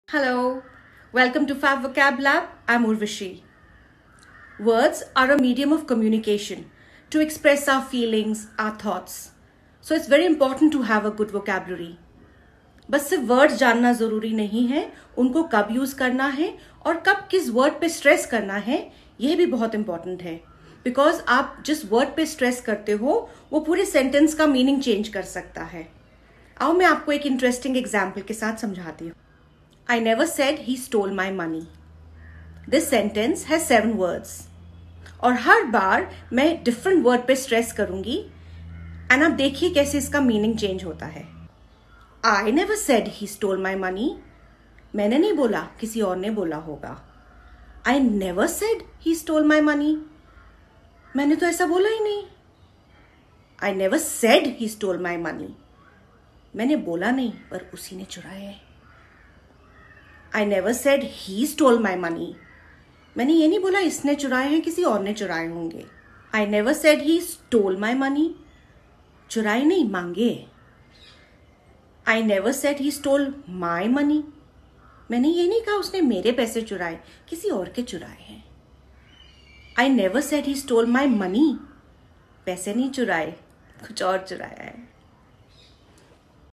How stressing on a word can change the meaning of a sentence.
By stressing the correct word, you make it easier for the listener to grasp the meaning of your sentence.